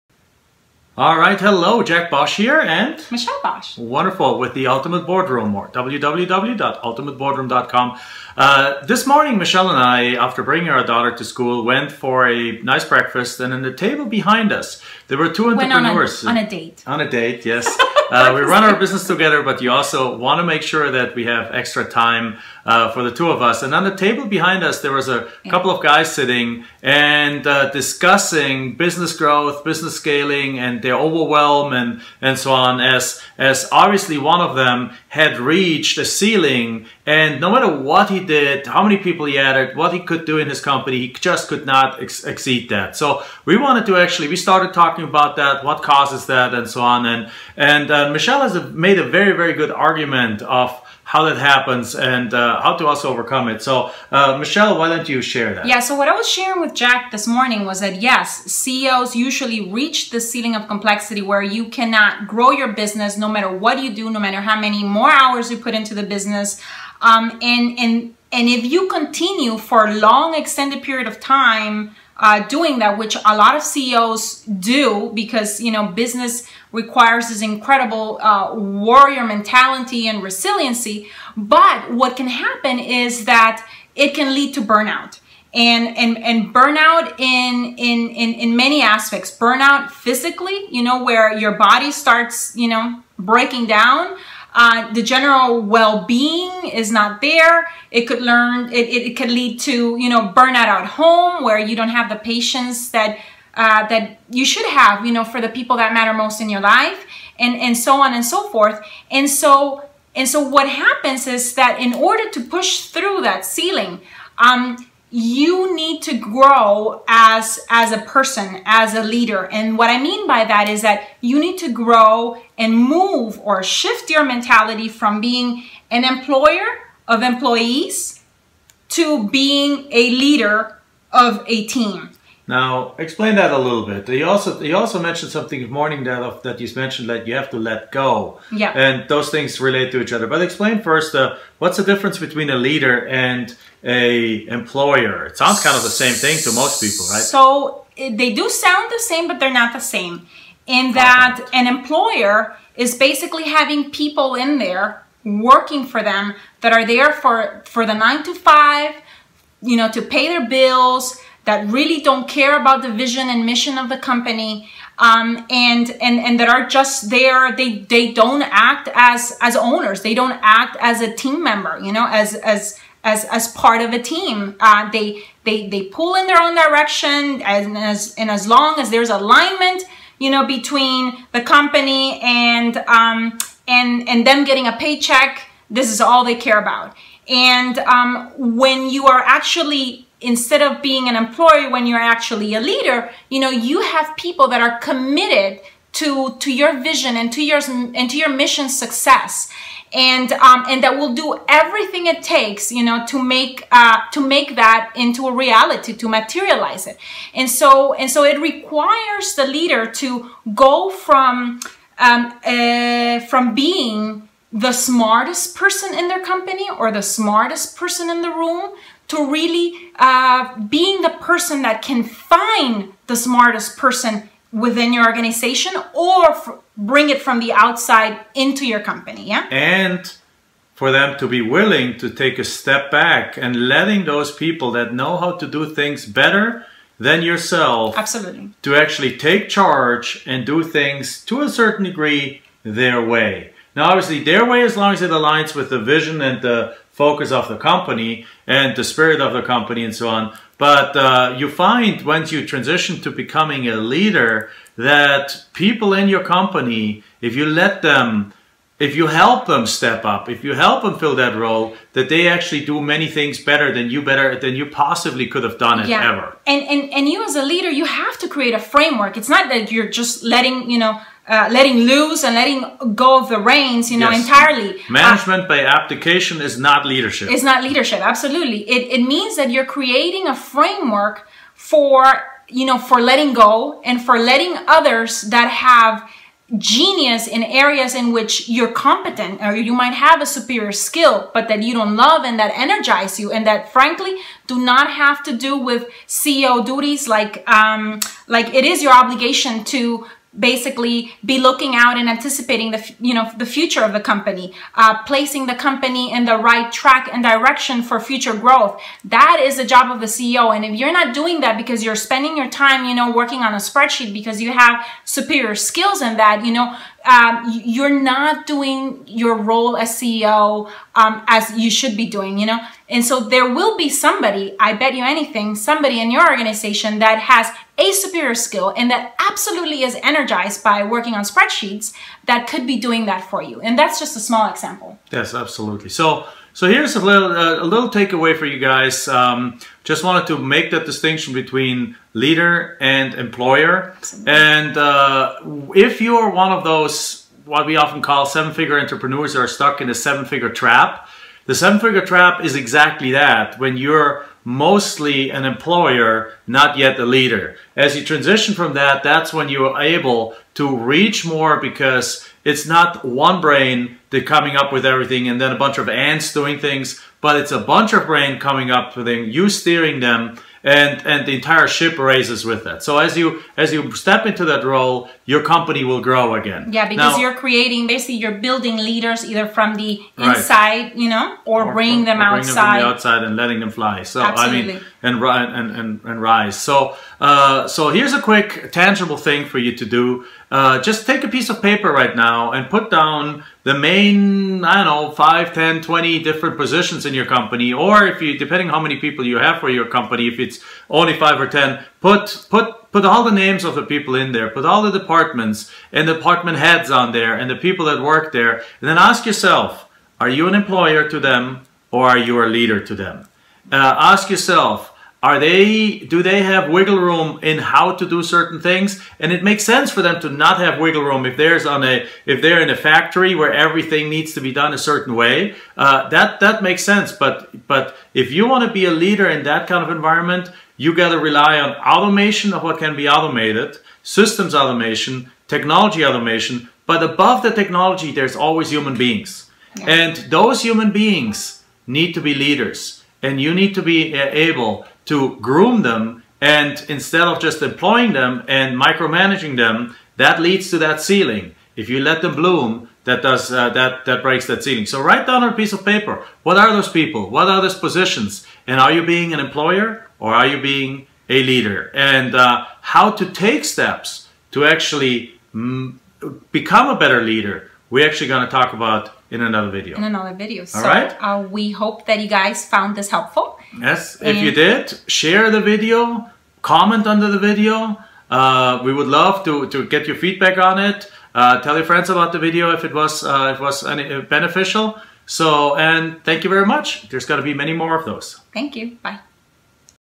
Are you an employer or a leader? Improve leadership skills with this quick business tip
- Michelle and Jack Bosch explain the huge difference between being an employer and a true leader of your company and business. Leadership skills are a must if you want to bring your company to the next level. Learn how to build and motivate a team around you that will allow you to scale your business up.